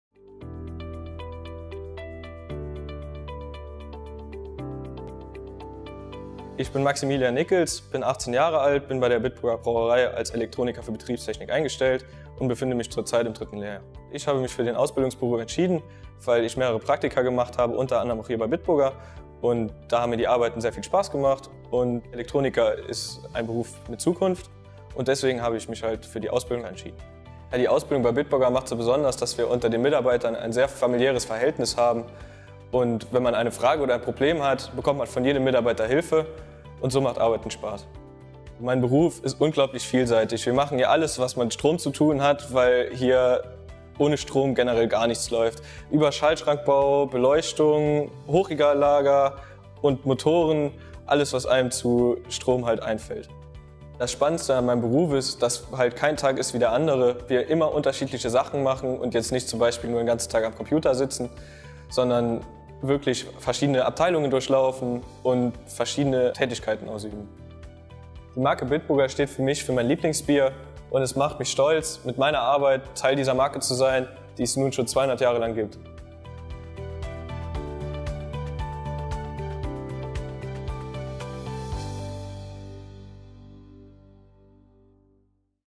Elektroniker für Betriebstechnik (m/w)
Eine gute Grundausbildung, Planung von Schalttechnik, Energieversorgung, modernes Engineering – das sind die Schlagworte mit denen man die Ausbildung unserer der Ausbildung zum Elektroniker (m/w) für Betriebstechnik beschreiben kann. Informiert euch in unserem Video über diesen Ausbildungsberuf bei Bitburger. 🍺🙂